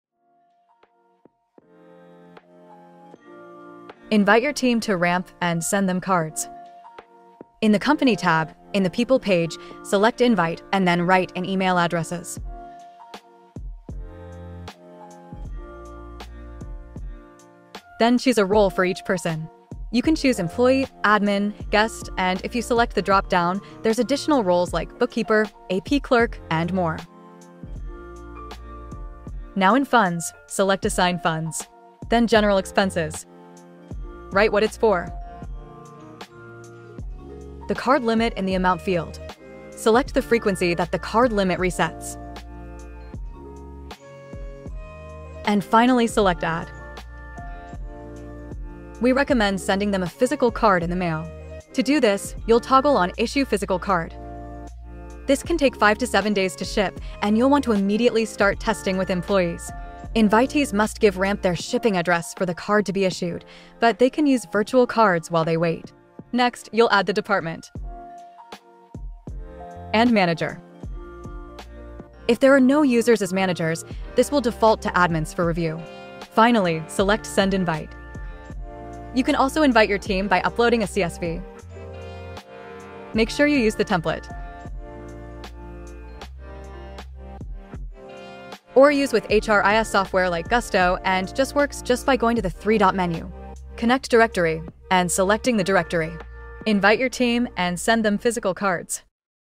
Invite your team to Ramp & issue them cards
Add users to Ramp and issue cards to start testing policies and real spending. You’ll learn how to assign roles, send physical or virtual cards, and get new teammates up and running quickly.

Not on Ramp yet? Learn more and schedule a live demo to see how Ramp can save your business time and money